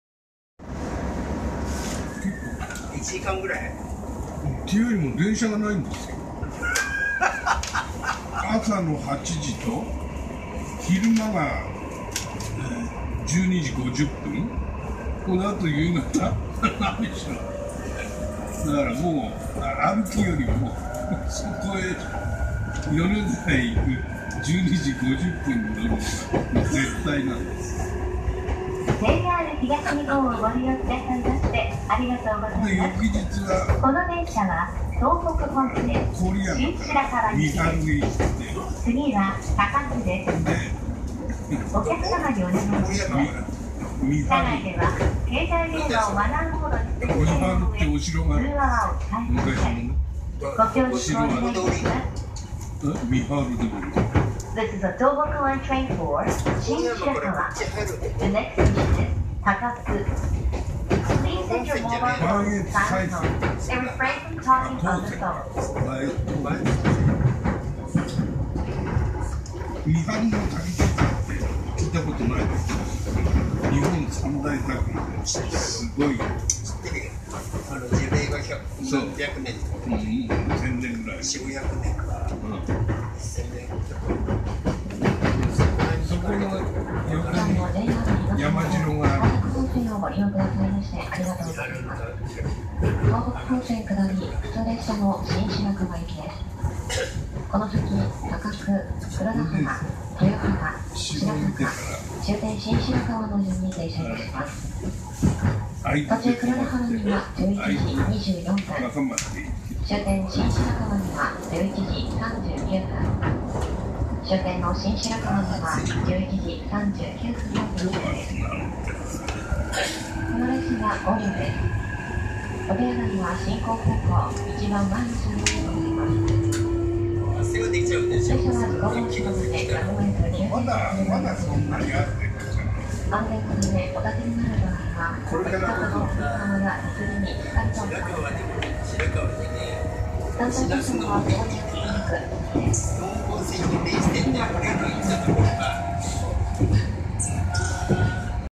【電気がないとすごく静か！】東北本線E531系 交直切り替え 黒磯駅発車後
JR東日本E531系3000番台による、JR東北本線 普通 新白河行の、黒磯駅発車後の直流→交流切り替えの様子及び車内放送です。
黒磯駅を発車してすぐの所には、架線に流れる電圧の直流と交流を切り替える設備があります。
通過している間は、車両に電流が一切流れなくなるため、E531系の場合は、通過時に冷暖房やモーターの音が完全に聞こえなくなります。
同区間を通過するキハ110系は、そもそも架線を必要としない気動車なので、直流と交流を切り替える設備を通過する時には、変化は何も起こりません。

直流→交流切り替え･･･0:45以降